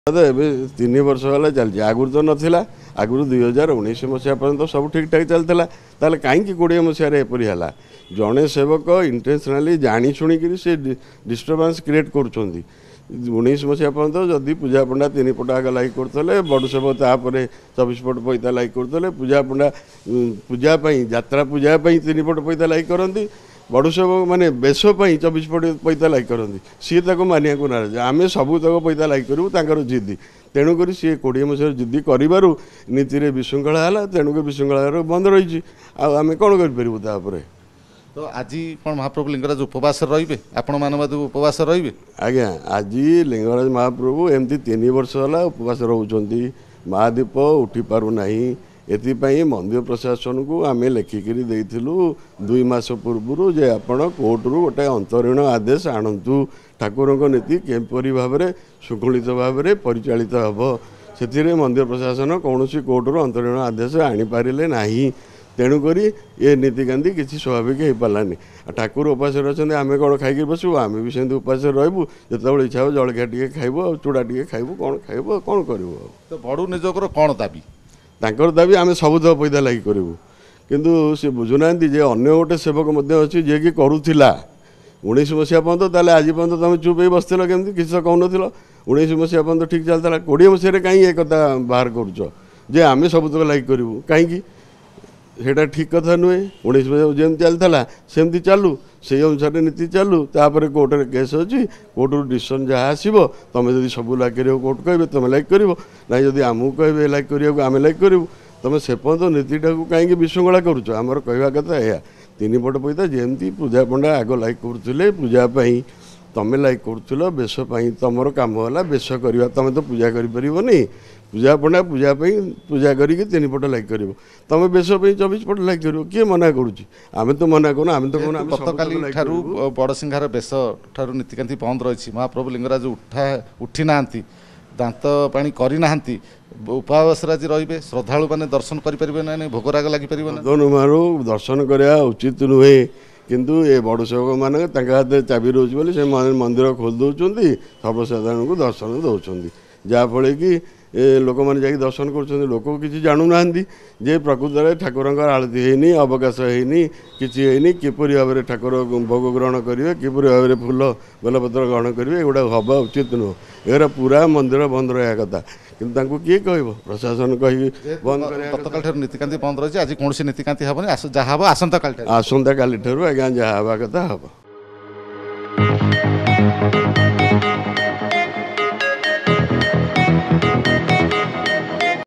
‘ଜିଦି ପାଇଁ ବିବାଦ ଲାଗି ରହିଛି’ | Odisha Reporter
ପଇତା ଲଗା ନୀତିକୁ ନେଇ ଦୁଇ ସେବାୟତ ଗୋଷ୍ଠୀଙ୍କ ମଧ୍ୟରେ ବିବାଦ। ଏହି ବିବାଦରେ ବଳି ପଡିଛନ୍ତି ମହାପ୍ରଭୁ ଲିଙ୍ଗରାଜ। ଗତକାଲି ବଡସିଂହାର ବେଶ ଠାରୁ ସମସ୍ତ ନୀତିକାନ୍ତି ବନ୍ଦ ରହିଛି। ସେପଟେ 2019 ପୂର୍ବରୁ ଏମିତି ବିବାଦ ହେଉନଥିଲା ଏବେ କେବଳ ହେଉଛି। ସେବାୟତଙ୍କ ଜିଦି ପାଇଁ ଏପରି ବିବାଦ ସୃଷ୍ଟି ହେଉଛି ବୋଲି କହିଛନ୍ତିି ପୂଜାପଣ୍ଡା ନିଯୋଗର ସମ୍ପାଦକ ସଚ୍ଚିକାନ୍ତ ପୂଜାପଣ୍ଡା।